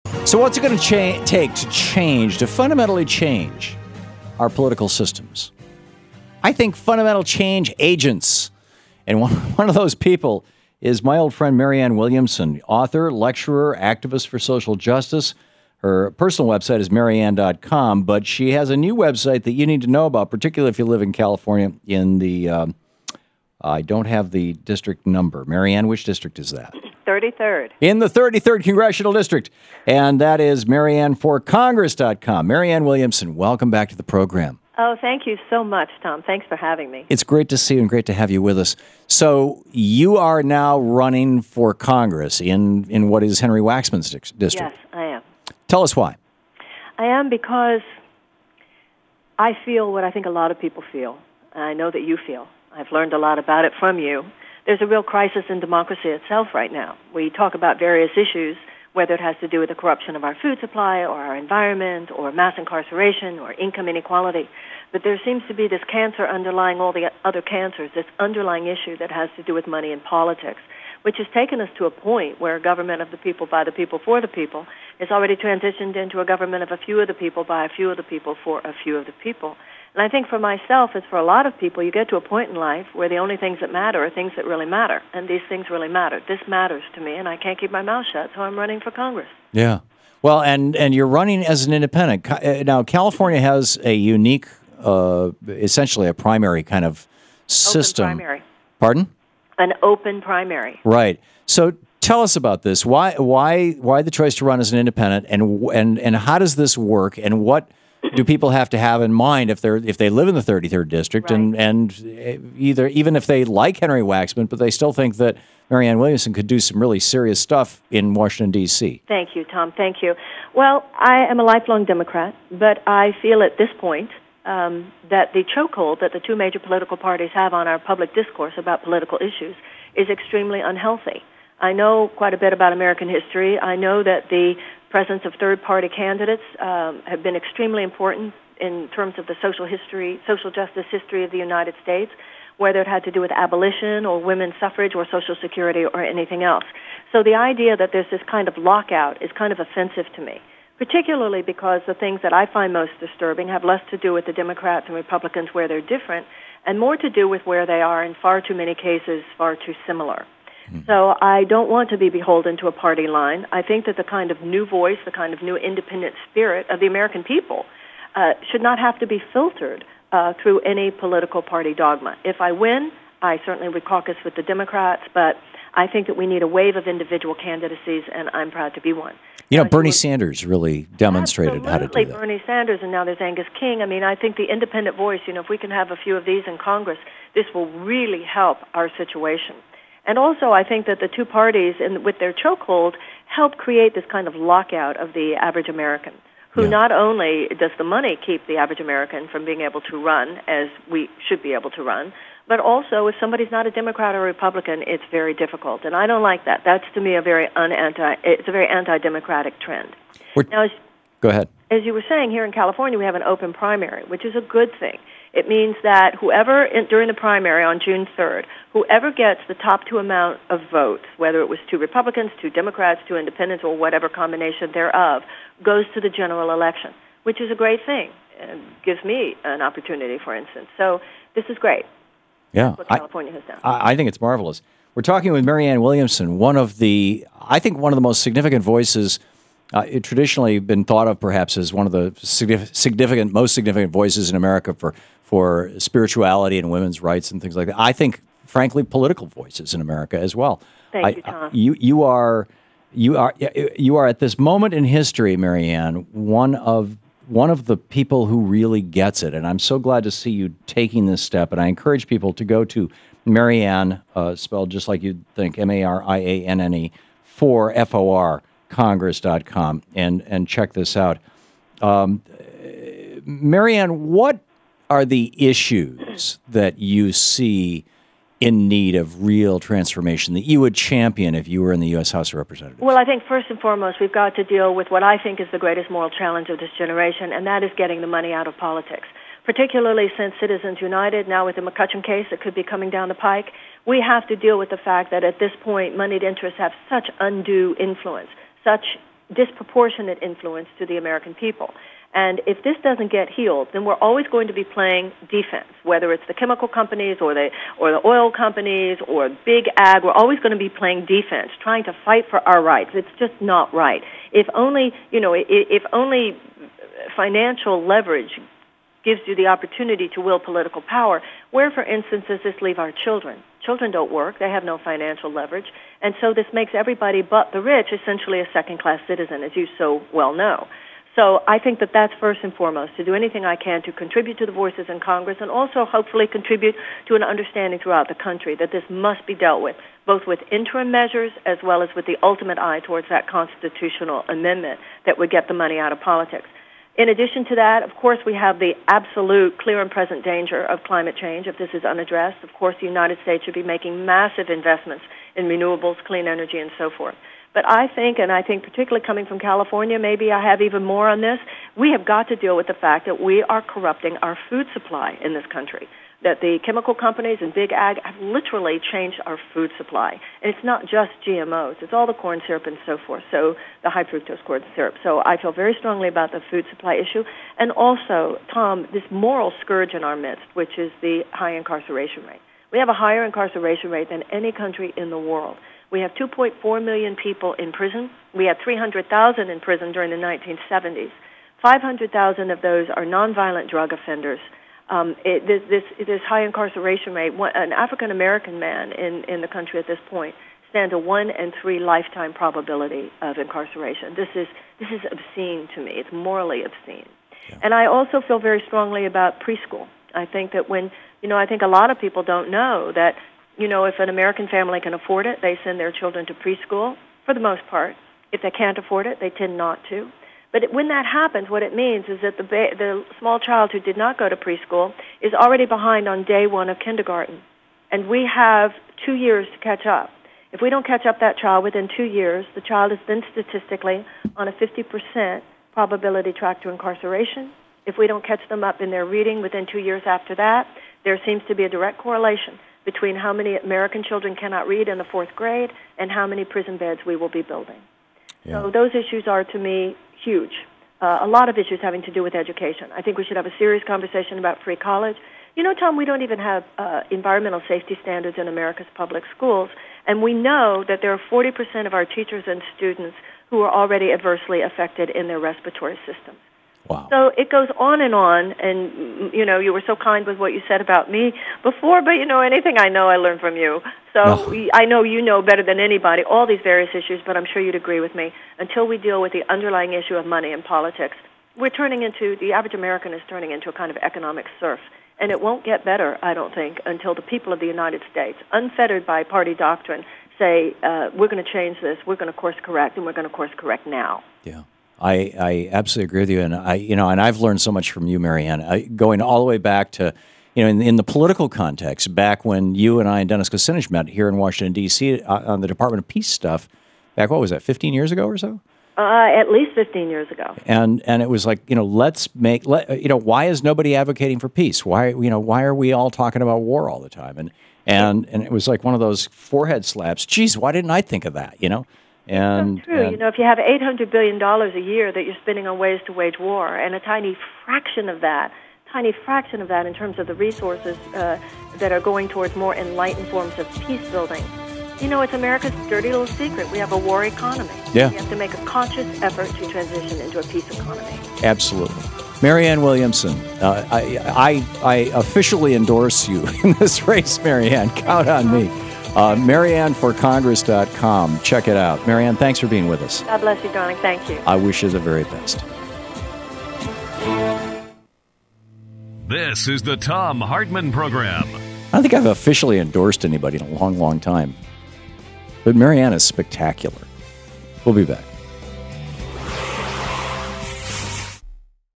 Why is Marianne Williamson Running for Congress?
Thom Hartmann talks with Marianne Williamson, author, lecturer and activist for social justice (personal about her Independent campaign against 38-year incumbent Congressman Henry Waxman in California.